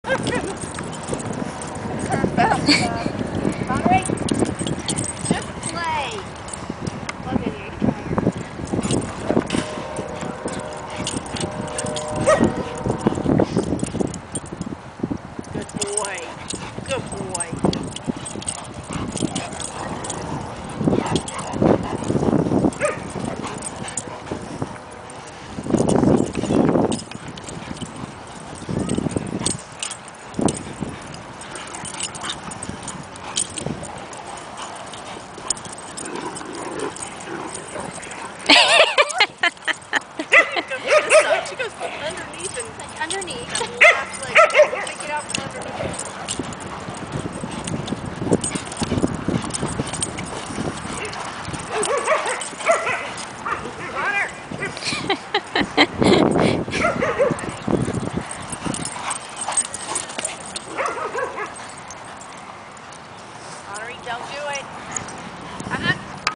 550+ Pounds of Great Dane/IW play!
550+ pounds of canine play at the dog park. Tank, Konner, Alliebug, and Colby are all friends!